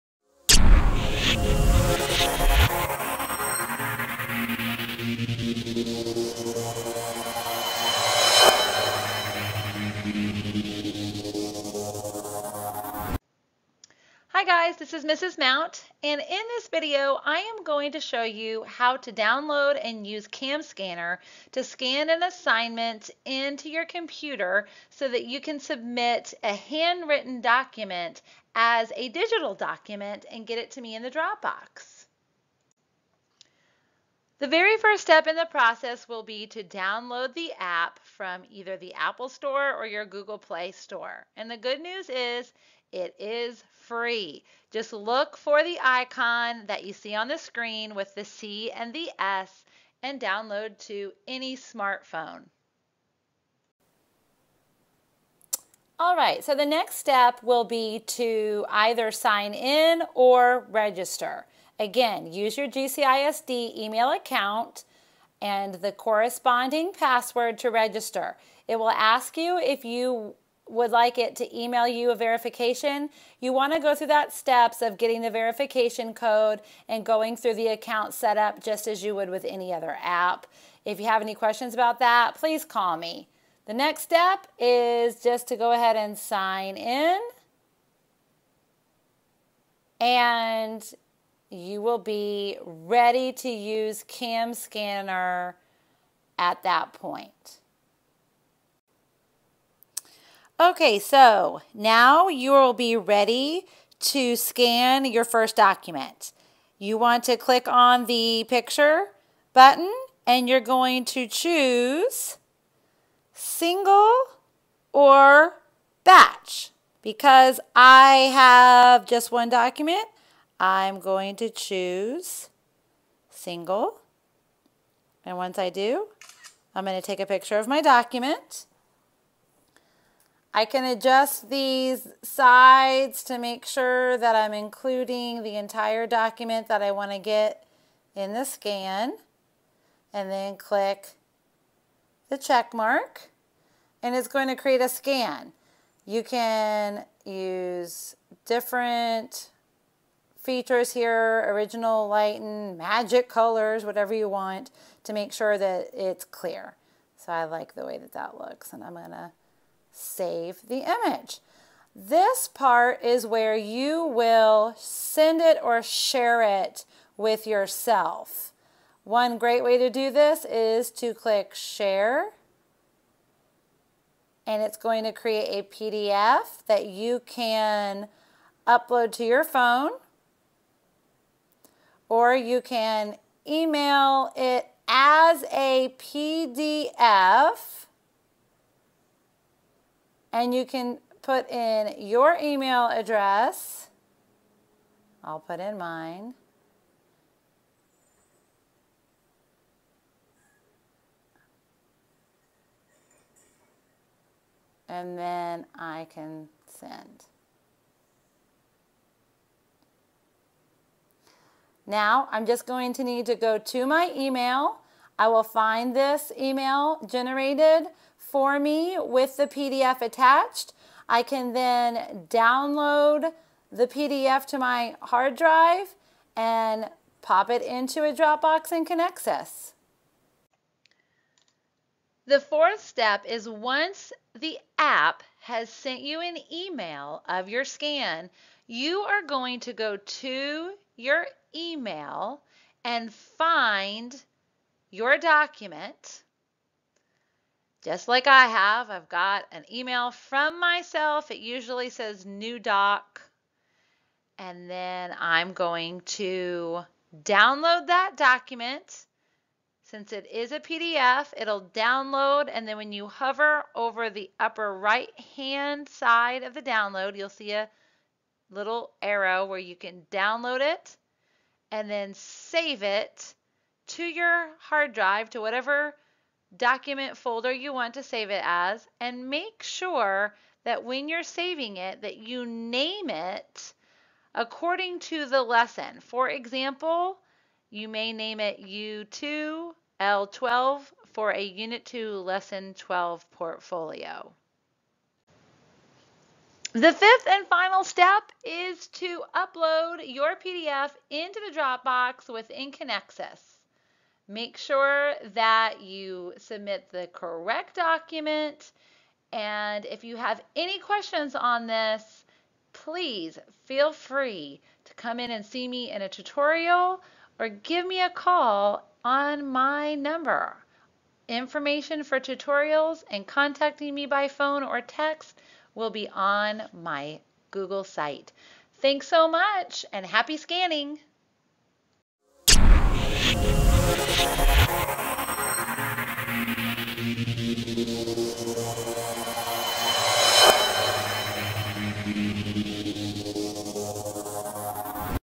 Using Cam Scanner to scan an assignment
A video to help students  use Cam Scanner to submit handwritten assignments as digital documents.